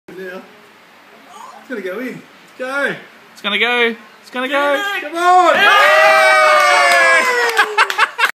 Bug sink pool ball
Bug on pool table sinks ball